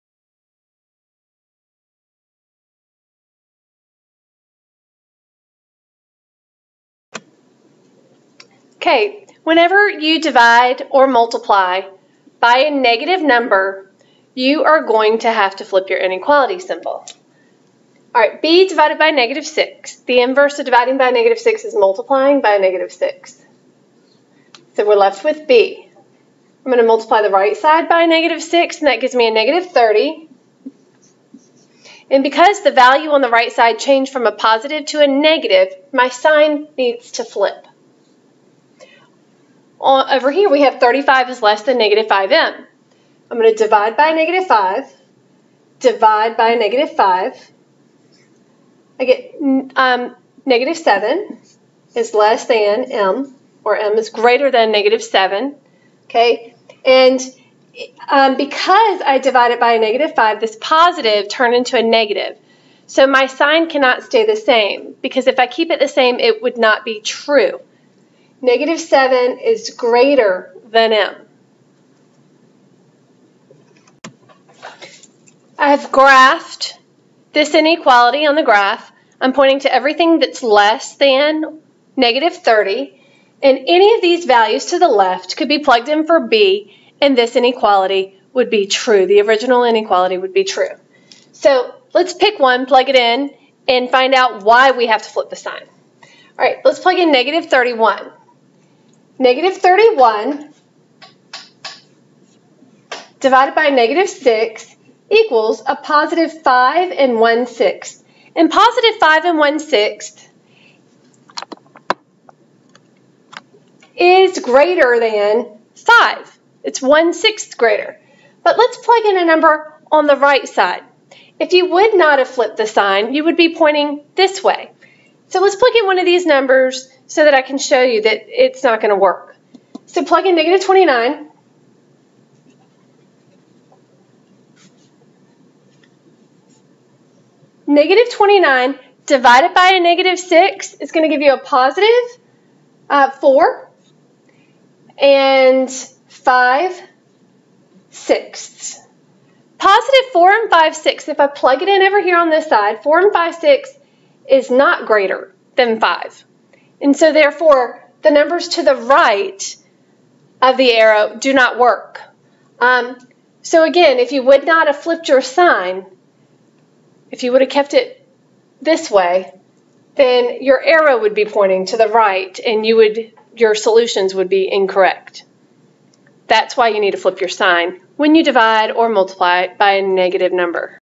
Flip The Sign: When do I flip the sign?
Learn why you would flip the sign in an inequality.